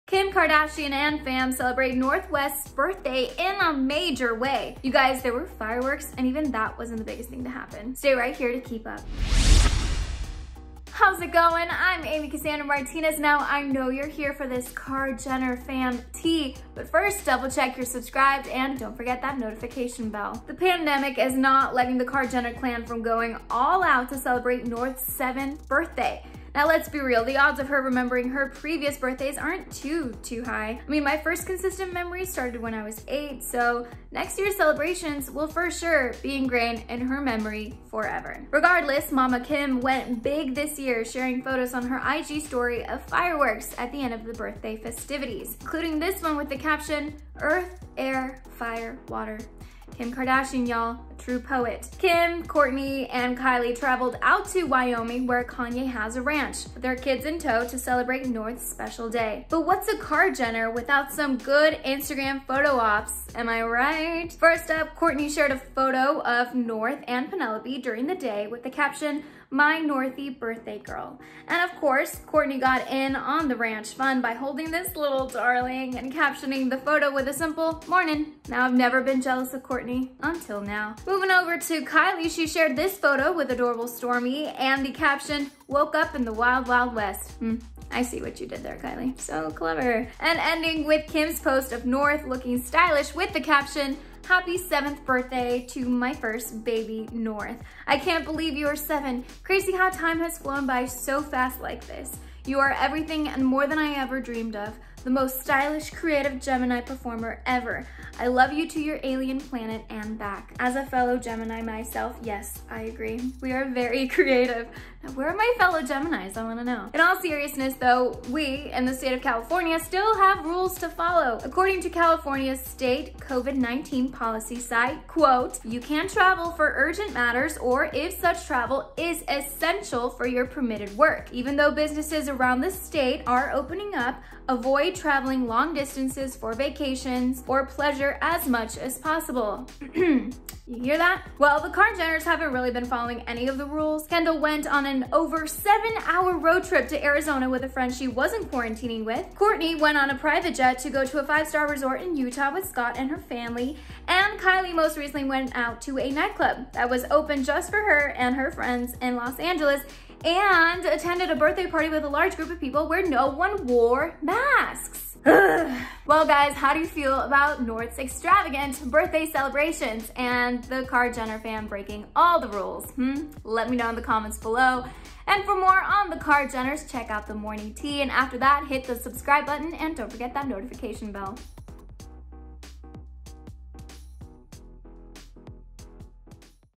Kardashian’s CELEBRATE North West’s Birthday At Kim &amp; Kanye’s Wyoming Ranch!
Kim Kardashian and fam celebrate North West’s birthdays in a MAJOR way. You guys, there were fireworks, and even that wasn’t the biggest thing to happen.
Stay right here to Keep Up.